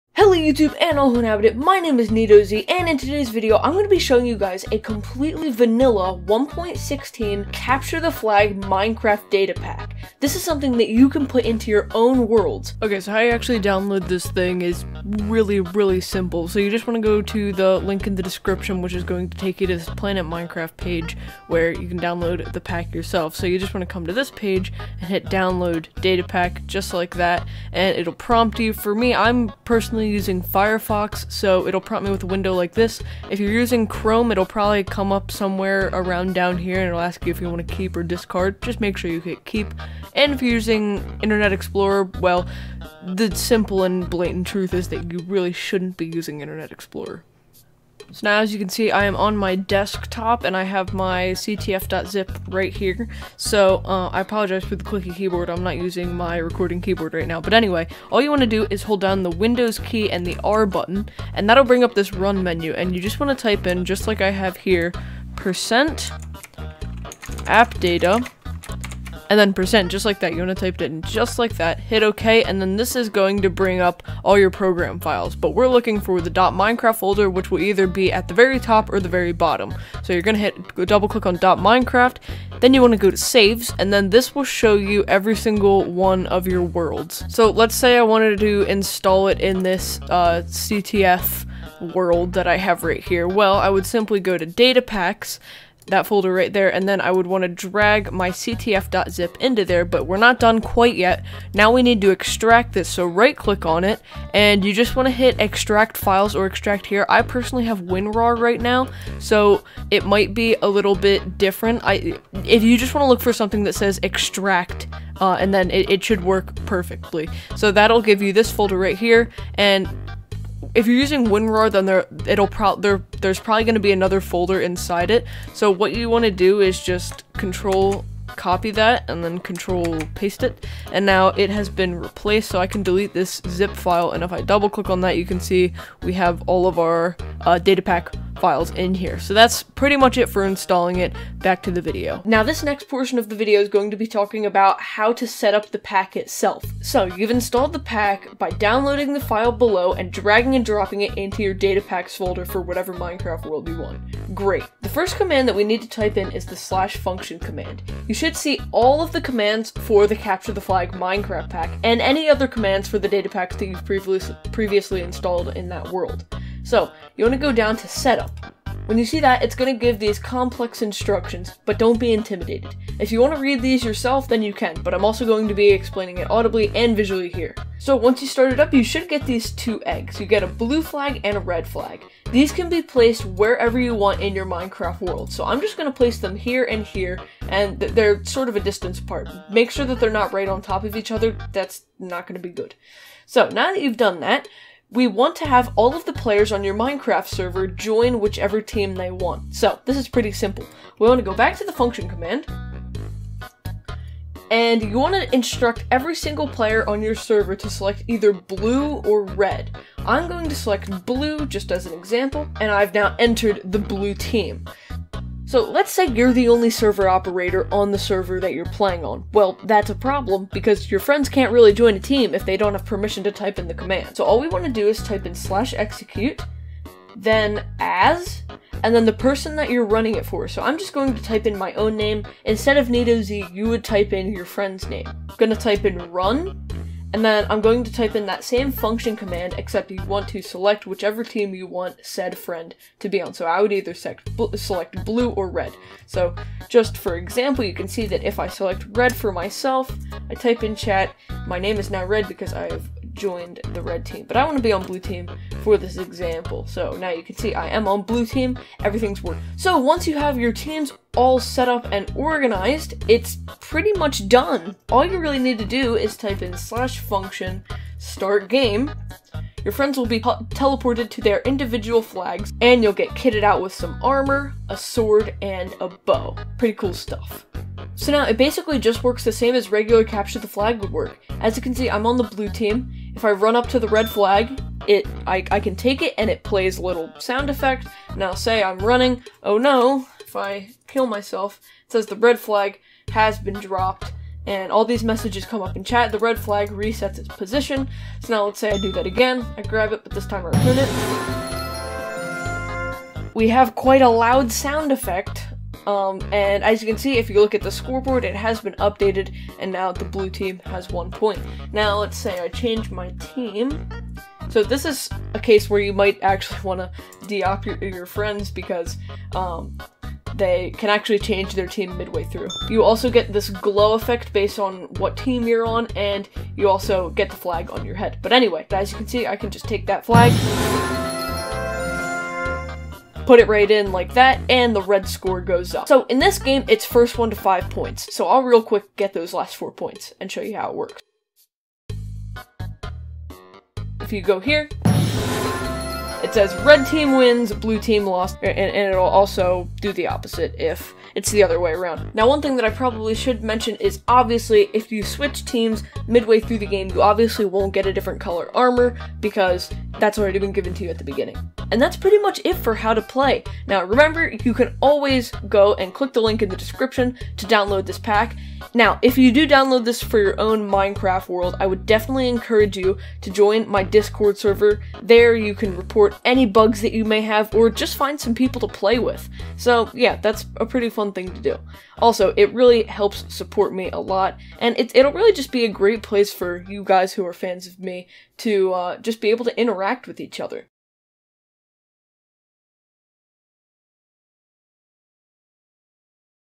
Vanilla Capture the Flag Data Pack [1.16]
In today's video, I show you all how to download and use my 1.16 vanilla Capture the Flag data pack. Download below. I also now have a discord server. You can report bugs, make suggestions, and just socialize with other fans. Let me know if you have any future suggestions for videos or if there are any ways that I can improve. Thanks!